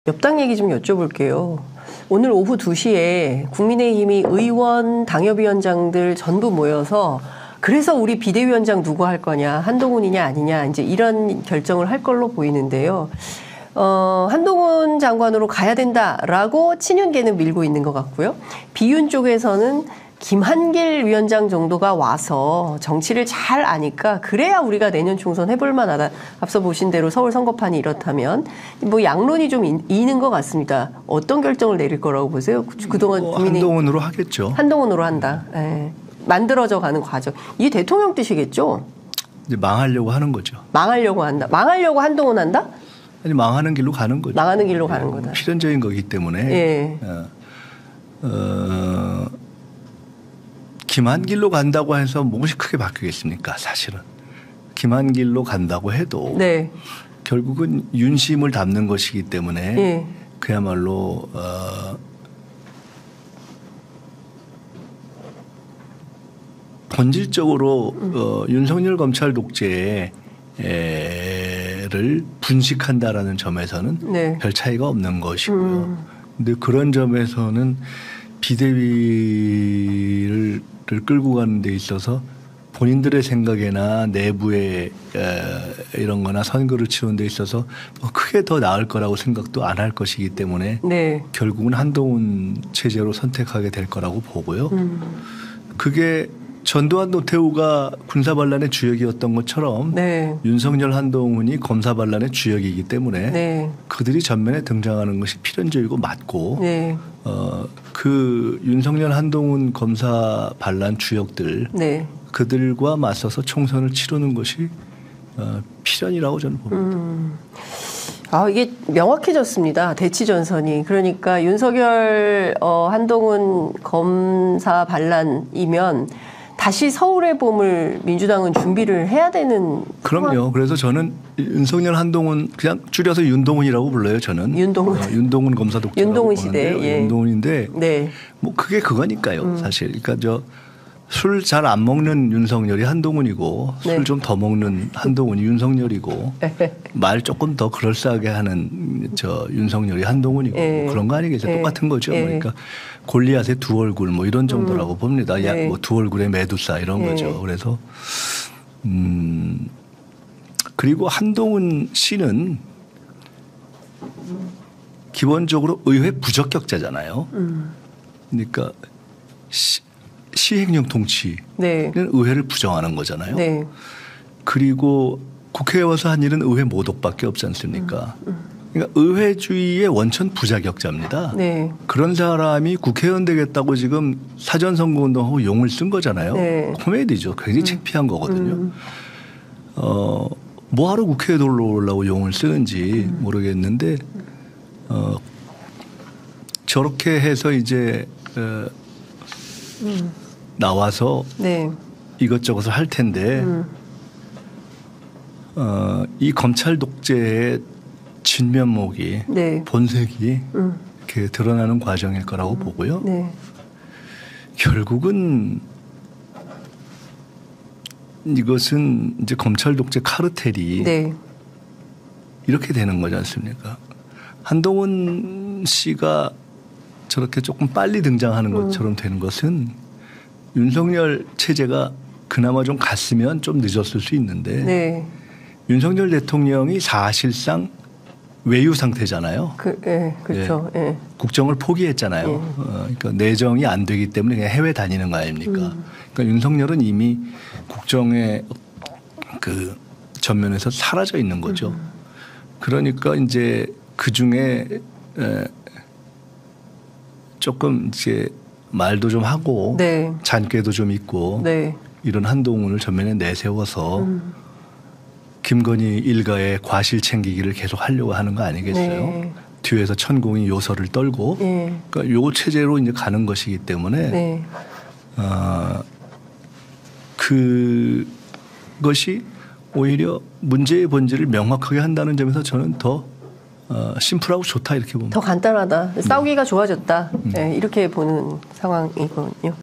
한동훈 비대위원장? 망하는 길 - 김민석 더불어민주당 의원 | 장윤선의 취재편의점
12월 18일 월요일 '불편한 인터뷰'에서는 김민석 더불어민주당 의원을 모셨습니다.

쿠데타 진압. 민주당 혁신만이 서울의 봄을 살릴 것이다
- 왜 민주당은 이낙연 신당 제압 못 했나?
- 2024년 봄, 검찰독재에 맞서 선거혁명 가능할까?
- 어설픈 화합과 침묵한 민주당, 무엇이 문제인가?
- 양비론과 절충주의로 이루어진 여의도 문법은 구시대 유물
- 국민과 당원 대변할 정치, 민주당의 혁신